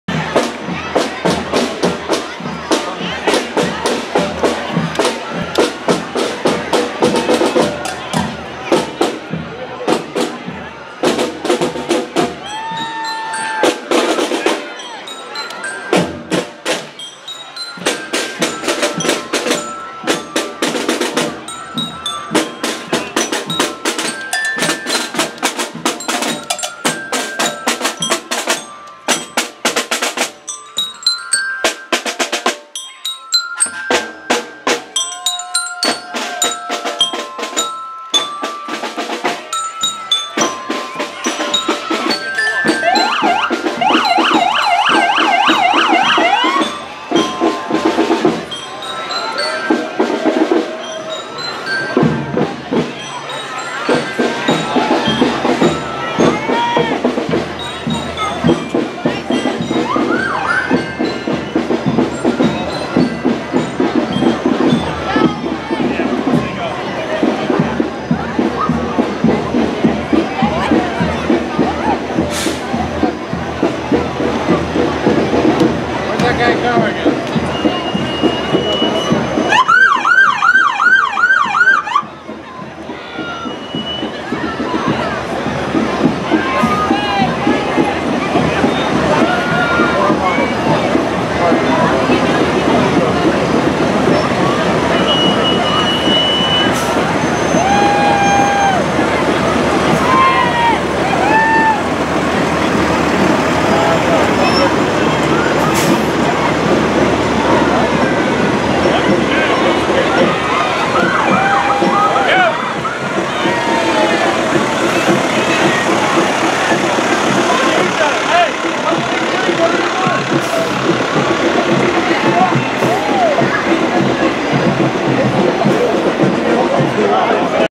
Greenlawn FD Parade 2019 - Part 9
Annual Greenlawn Fire Department Parade held on August 29, 2019